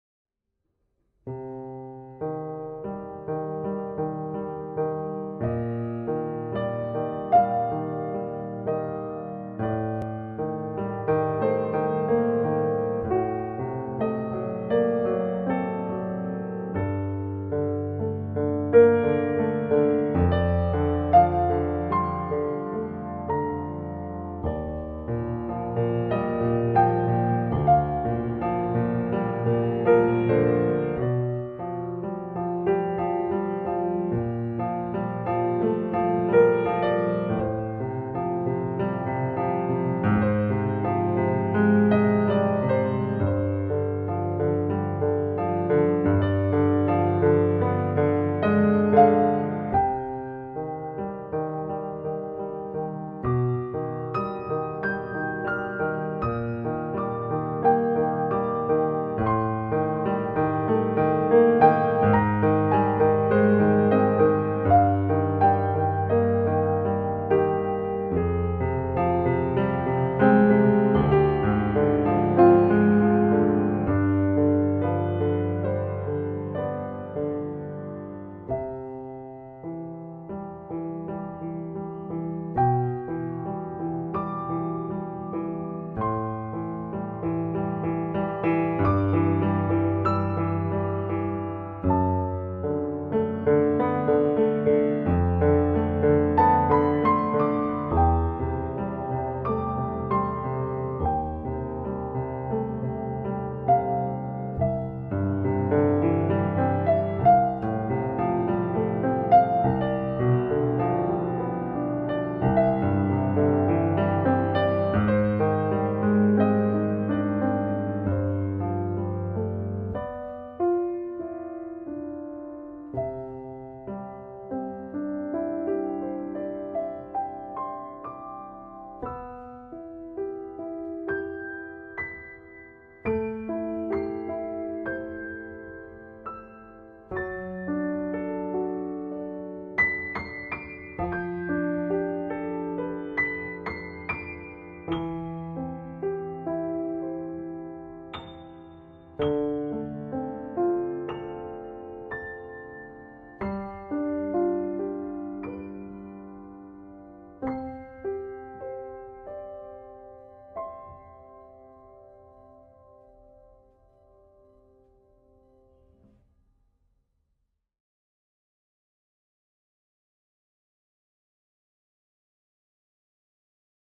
Giovanni Renzo_Candidatura House Concert - Piano City Napoli 2020
Composizioni originali ed improvvisazioni per pianoforte solo